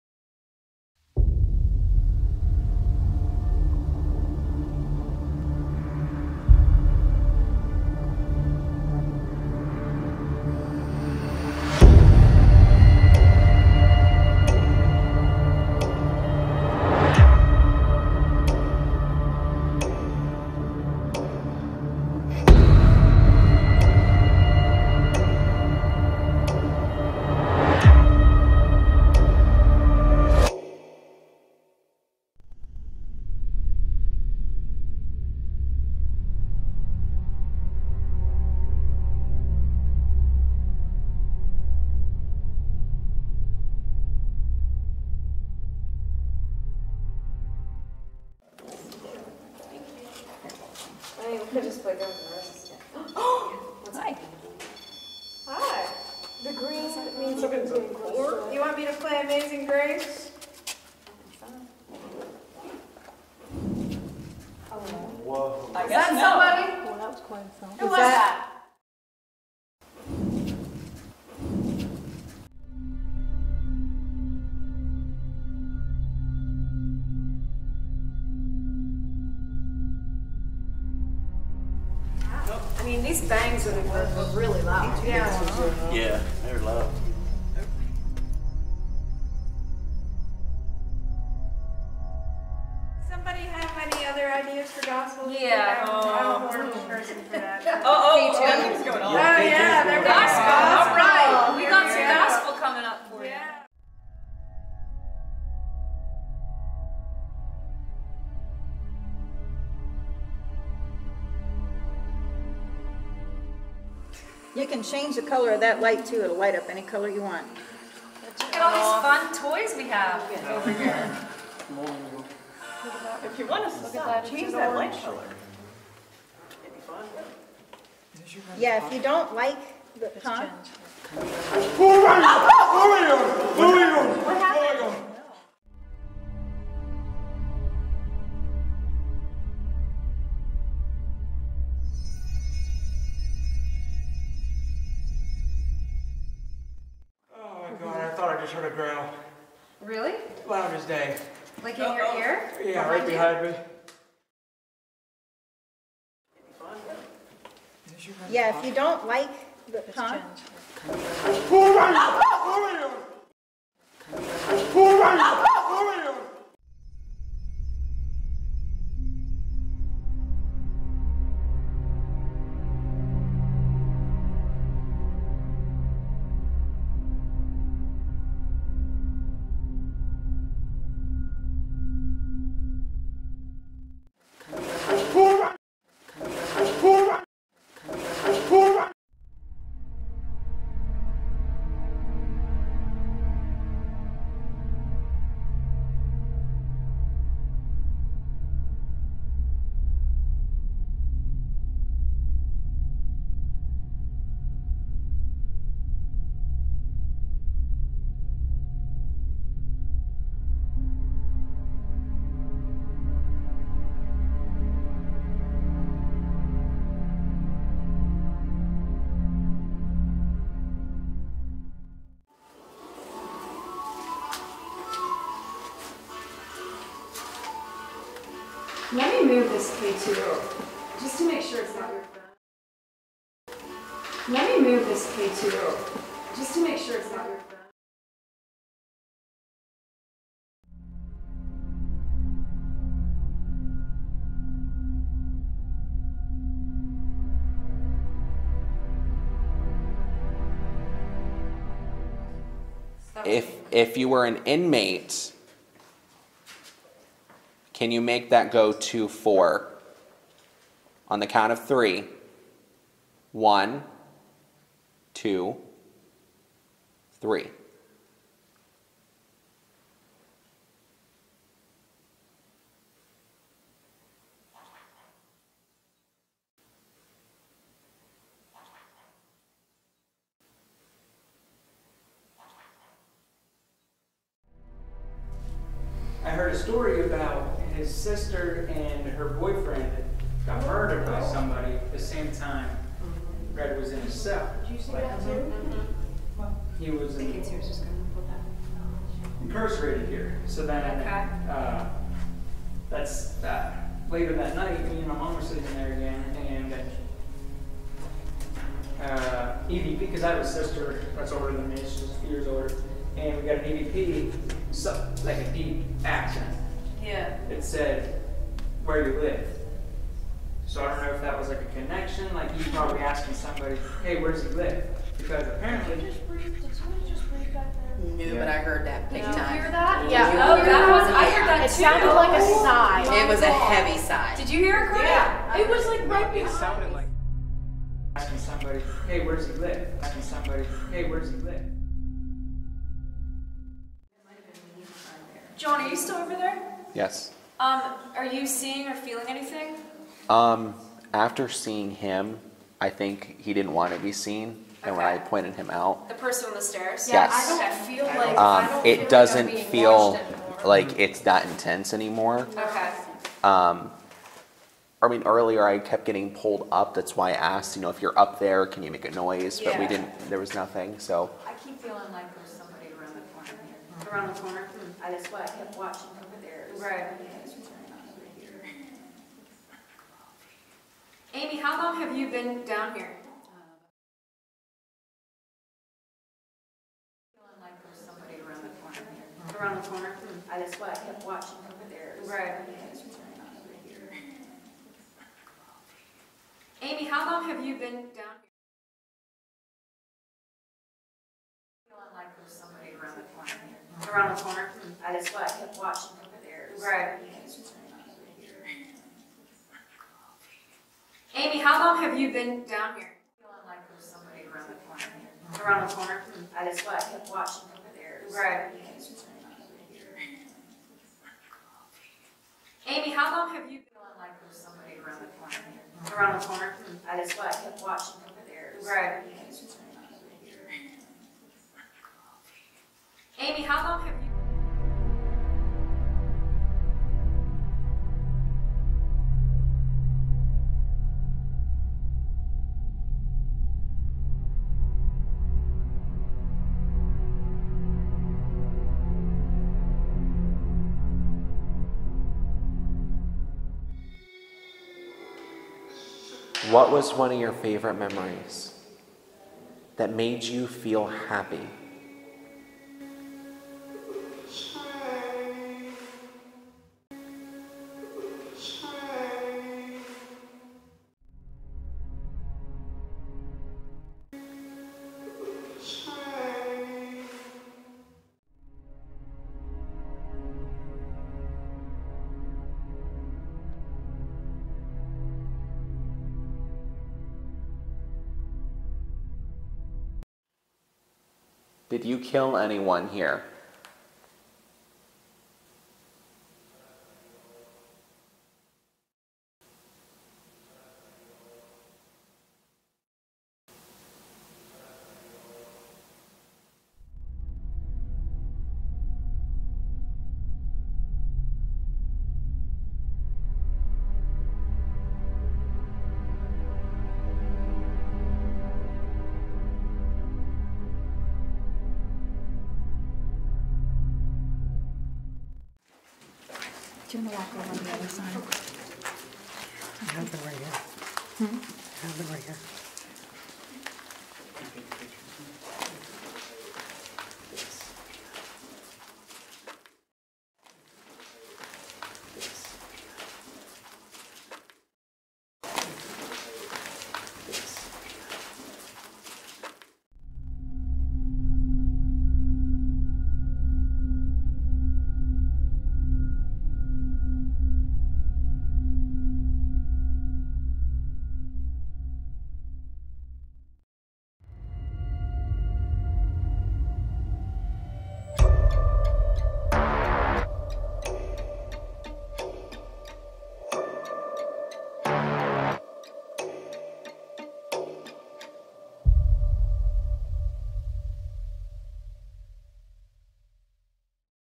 Evidence | West Virginia Penitentiary Paranormal Investigation
On August 13th, 2022, we investigated
the West Virginia Penitentiary with Paranormal Quest, Heather Taddy,
and guests as part of the West Virginia Penitentiary Para-Con.

Going through the most active areas... we recorded
audio the entire investigation.